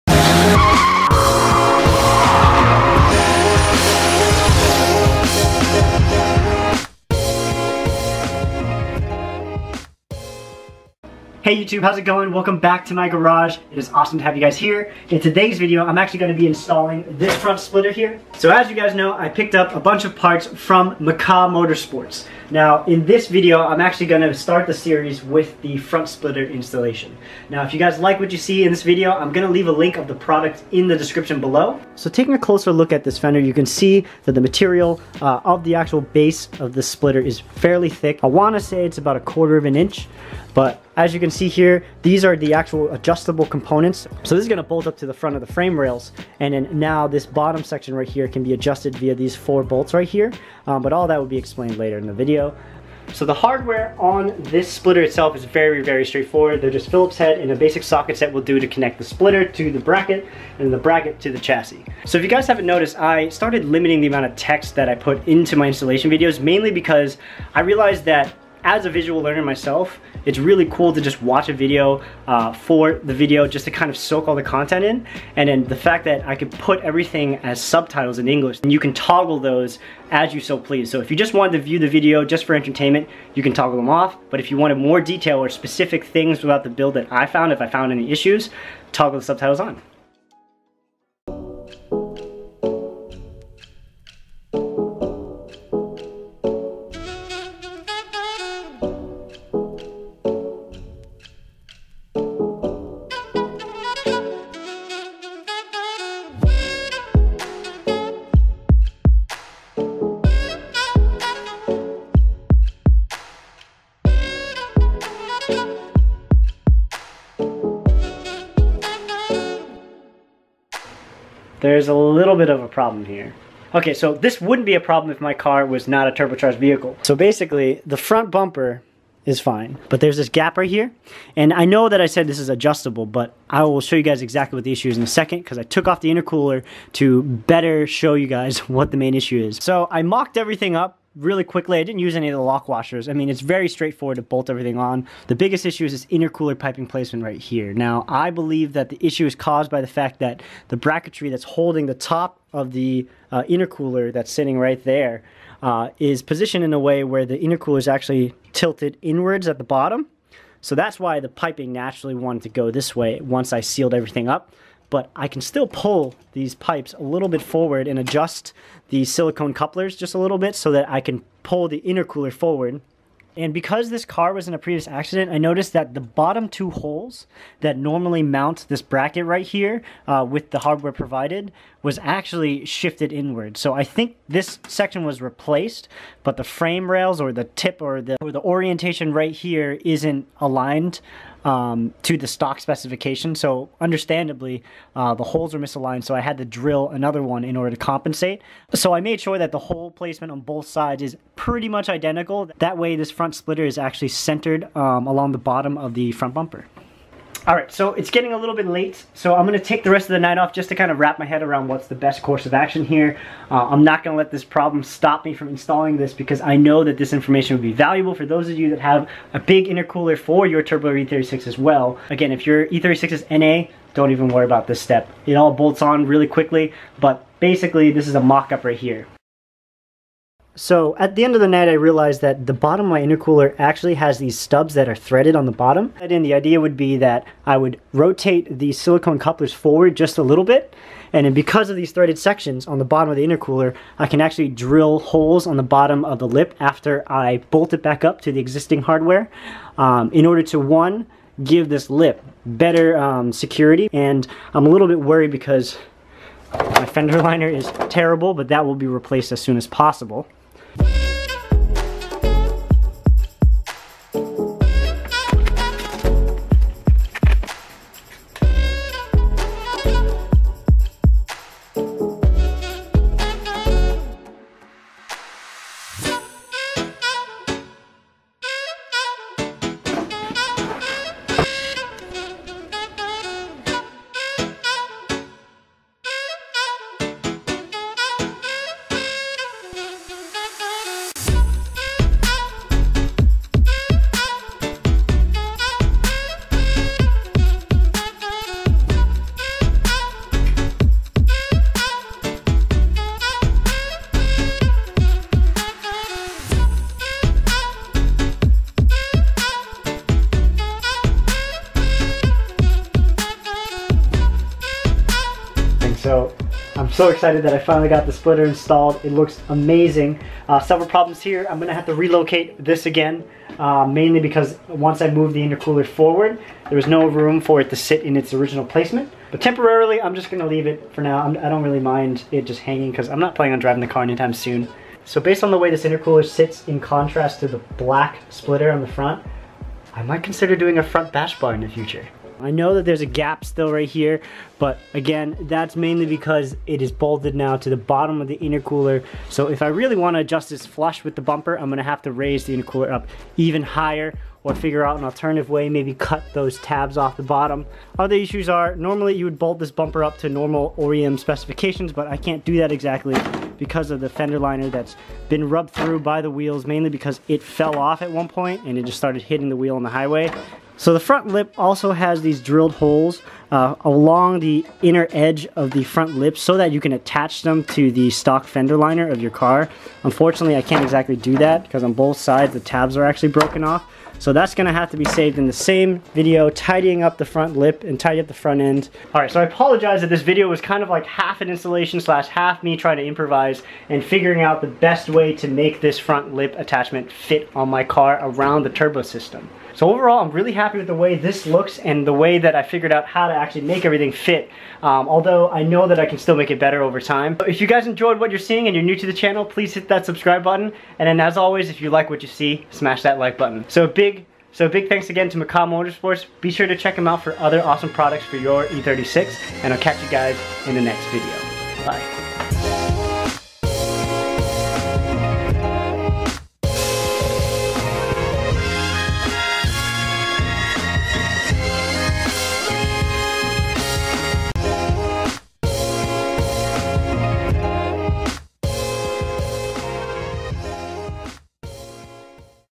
Will It Fit? Adjustable Front Splitter Install On My BMW E36 [MKAH Motorsports Front Splitter]!
In this episode I install the MKAH Motorsports adjustable front splitter (racing front lip)! This product came with easy-to-mount hardware, and a great glossy wrinkled finish. The biggest challenge was actually adjusting my aftermarket intercooler piping (N/A E36's don't need to worry about this issue), but with some small adjustments I was able to move the intercooler forward and make everything fit (minus the oil cooler and atrocious looking fender liner...those will be fixed eventually).